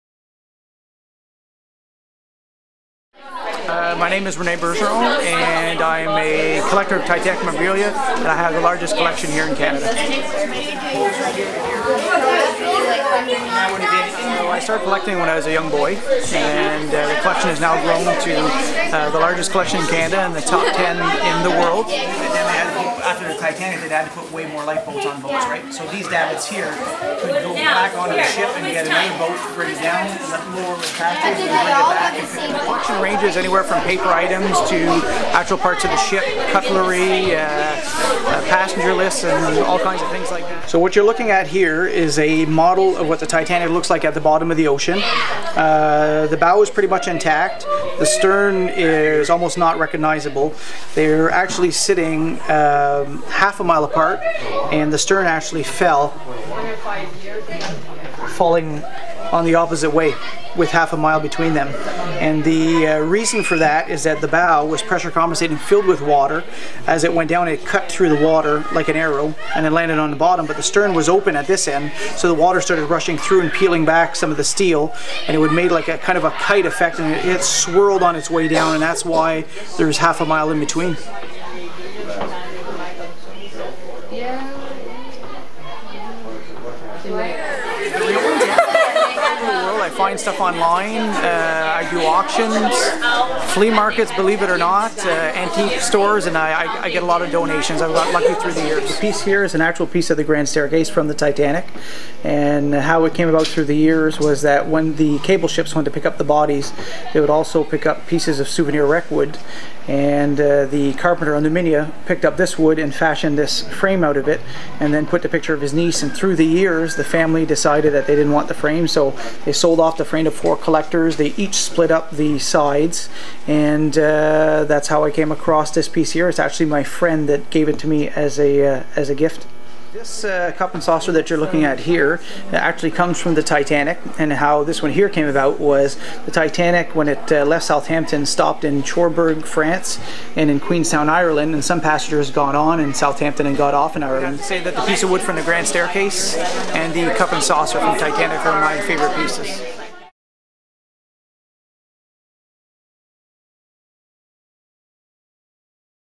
Rene Bergeron's Titanic artifacts collection
Rene Bergeron has the largest collection of authentic Titanic artifacts in Canada.